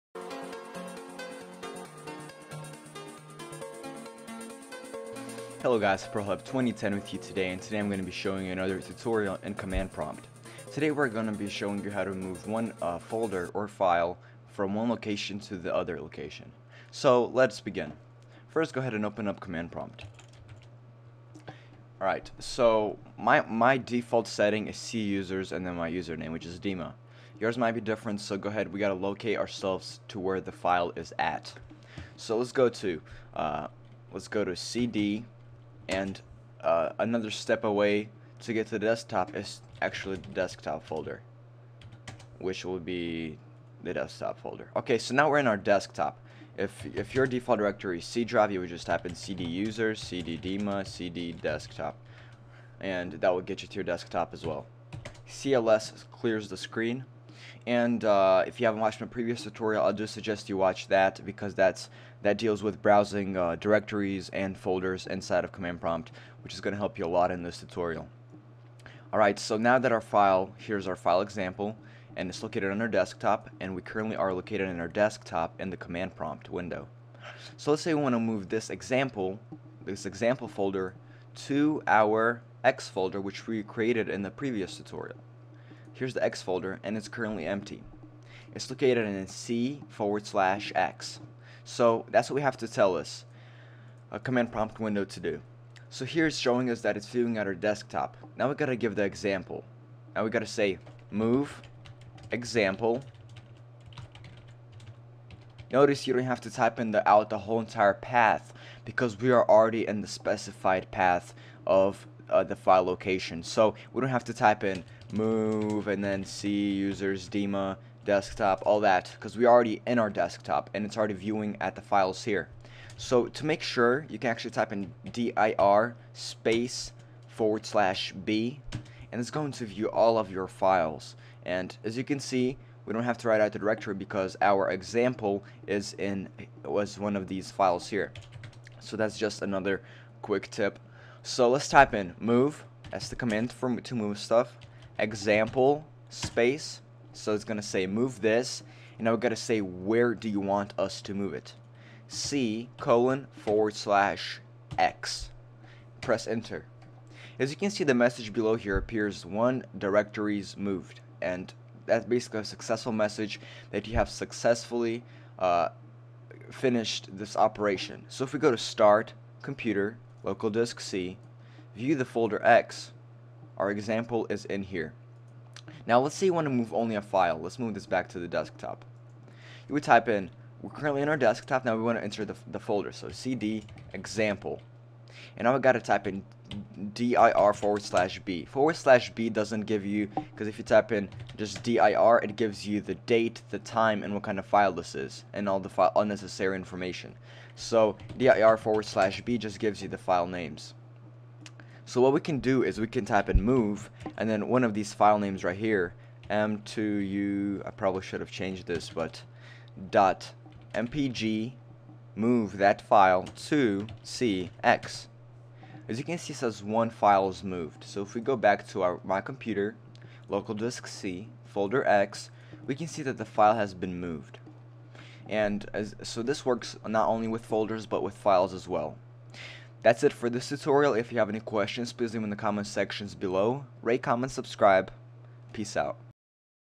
Command Prompt Basics-Moving Files and Folders in CMD [Tutorial 2]
This is how to move files and folders from within CMD.
It's pretty basic

the command you need to learn is : move

This is the setup

C:\yourfiledirectory  nameofFILE.fileextension C:\newdirectory

Tutorial 2 by prohelp2010